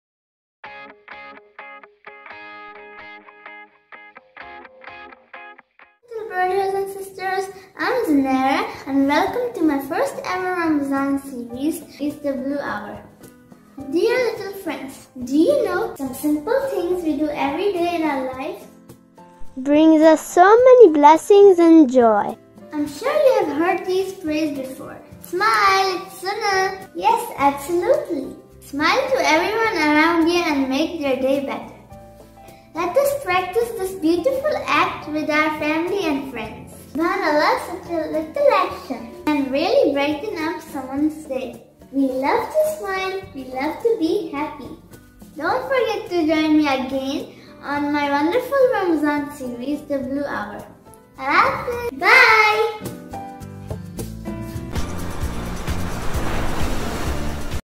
"Smile to Everyone" Zunaira's The Blue Hour Ramdan Series_ Episode 1
Zunaira Explains the importance and beauty of Smile in life. she shares beautiful messages through her Ramadan series " the blue hour"